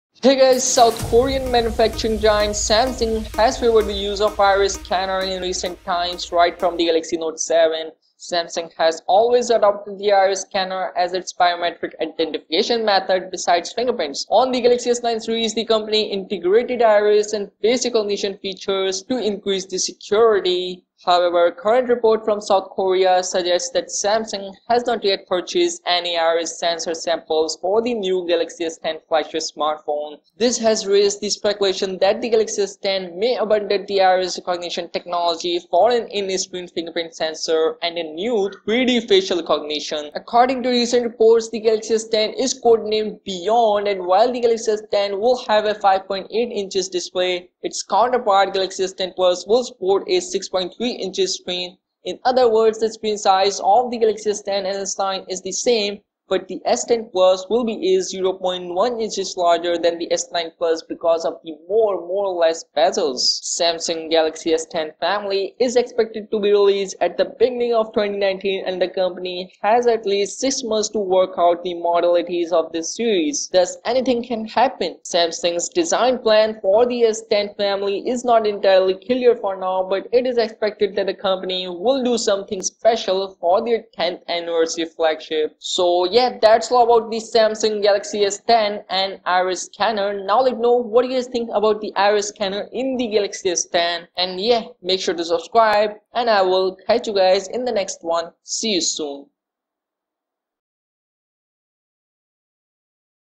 Galaxy S10 - Iris Scanner Big News!!!
Samsung Galaxy​ S10 May DITCH THE IRIS SCANNER FOR AN ON-SCREEN FINGERPRINT SENSOR.